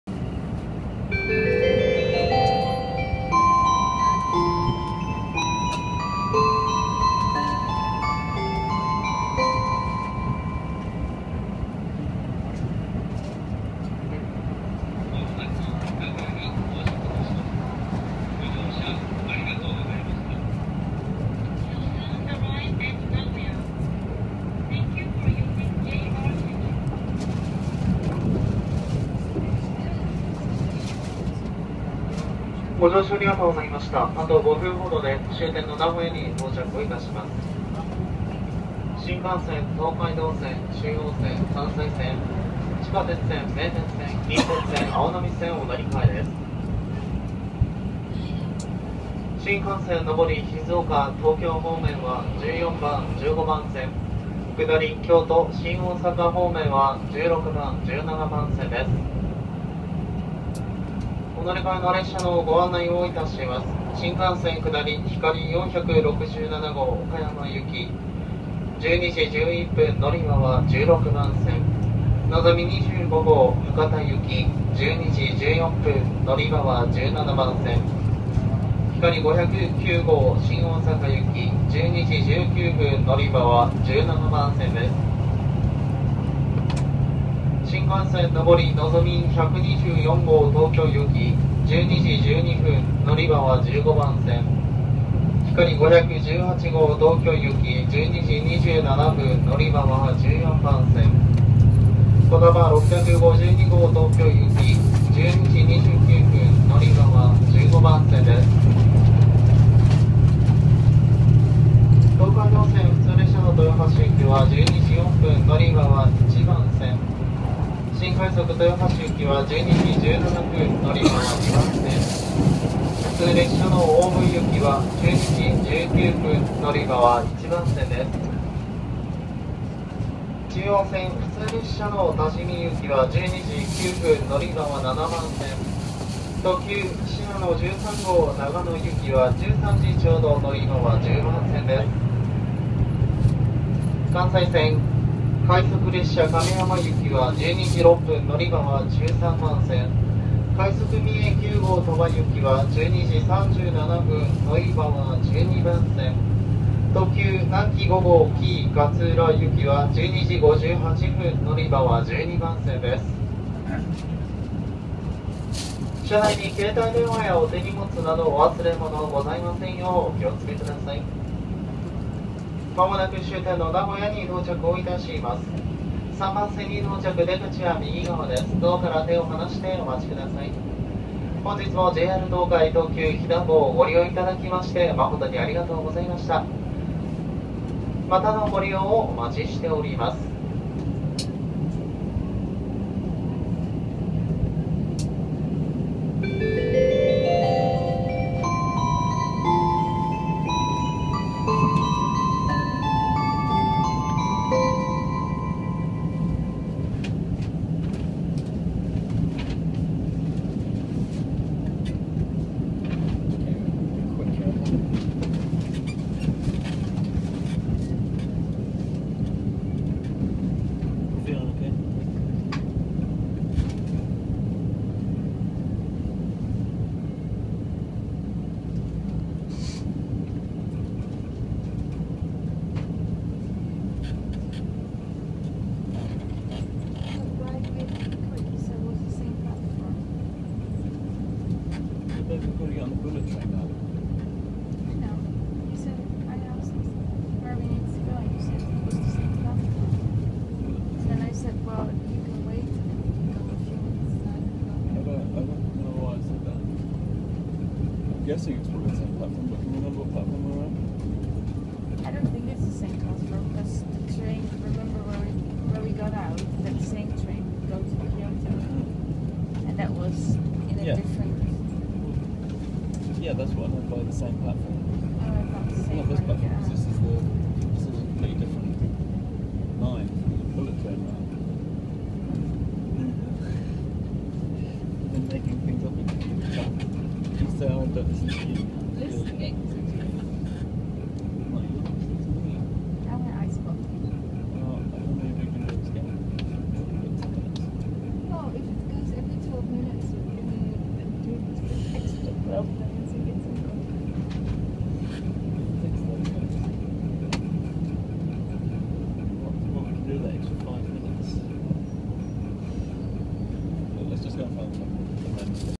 ワイドビューひだ 名古屋到着車内放送
ワイドビューチャイムで始まり、ワイドビューチャイムで締めます。